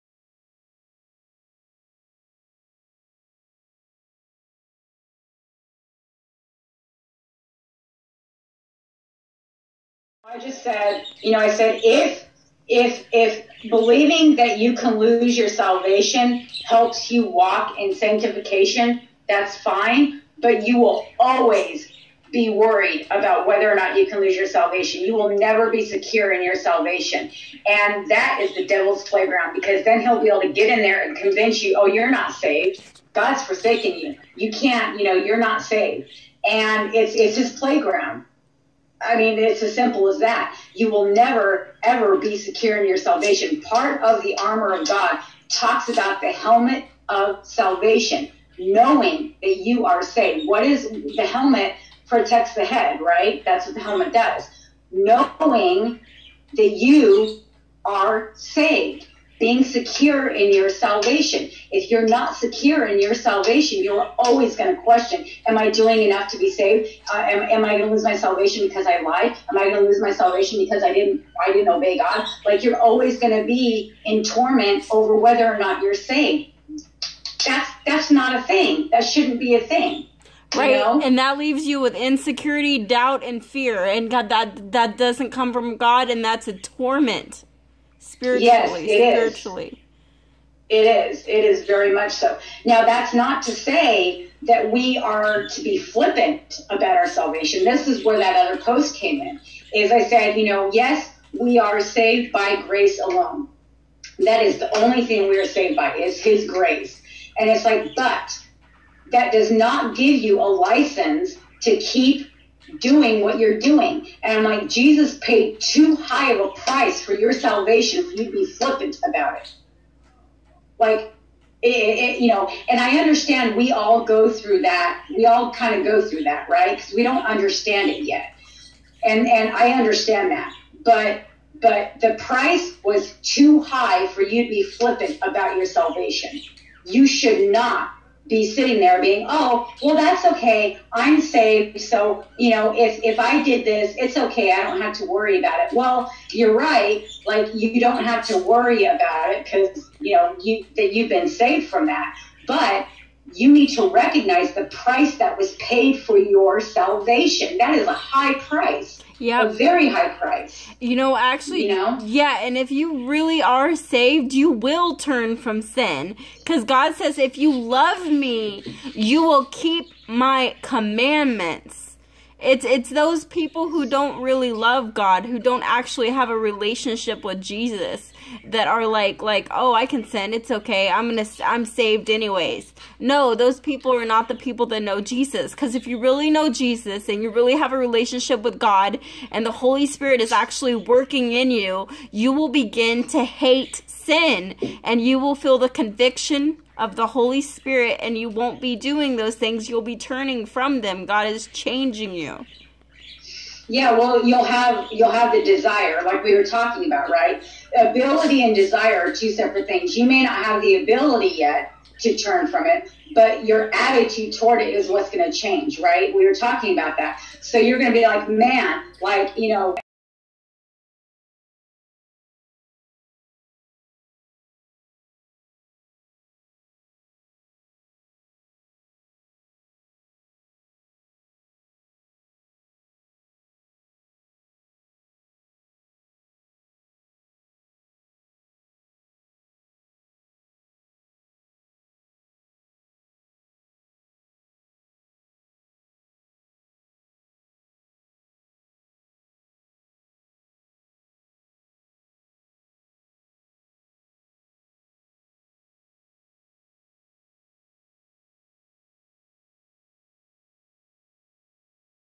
Helmet of Salvation sanctification Christian discussion
My friend Elizabeth were in a deep holyspirit filled conversation as we lovingly and passionately discussed biblical truth regarding the helmet of Salvation. It was so amazing that night Chickadee. Ty for giving me permission almost 2 yrs ago to post this to youtube. I love you. I pray the Lord uses this video for those being tourmented by satans lies. In Jesus name Amen